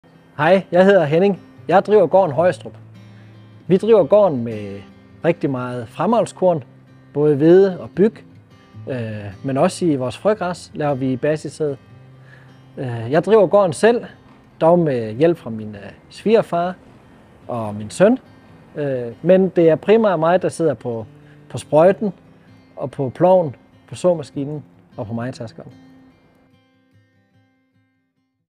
Follow a Farmer - Henning Madsen
Ikke langt fra kysten, ud mod Øresund, på den østlige del af Sjælland, ligger bedriften Højstrup, hvor Henning Madsen står for den daglige drift. Efter en række år med glidende generationsskifte, blev bedriften endeligt overtaget i sommeren 2021. Produktionen er ren planteavl på 265 ha, med fokus på fremavlskorn. Hertil udlejning af erhvervsejendomme. Ingen andre end Henning er involveret i den daglige drift, men svigerfar hjælper til når der er brug for det, og i høstsæsonen kommer ekstra hjælp udefra. Målsætningen for Henning er klar – gode afgrøder på alle hektarer, ud fra de forudsætninger jord og vejr giver.

I alt dyrker Henning hvede (80 ha), græs (65 ha), byg (70 ha), raps (30 ha) og hvidkløver (20 ha) på markerne.

Filosofi
Gode afgrøder på alle hektarer, og en kontinuerlig stræben efter at gøre det rigtige, ud fra de forudsætninger jorden og vejret tilbyder. Det er rettesnoren for Henning, der driver hans engagement, når ukrudtet i markerne kræver mange timer – på Højstrup tolereres ukrudt ikke.

Henning er med i Follow a Farmer for at dele sin viden, sætte fokus på det han laver og give inspiration til andre i faget.

Bedriften
På Højstrup driver Henning Madsen næsten gården helt på egen hånd, kun med hjælp fra svigerfar i perioder. Henning bruger meget tid på kampen mod ukrudtet i marken, så udbyttet af fremavlskornet bliver optimalt. Hertil kommer udlejninger af erhvervsejendomme og en mark med blomster, der sælges fra en vejbod.

Afgrøder
På de i alt 265 ha, dyrker Henning bl.a. fremavlskorn og har hvede (80 ha), græs (65 ha), byg (70 ha), raps (30 ha) og hvidkløver (20 ha).

Produkter
På Højstrup anvendes og testes forskellige BASF – produkter.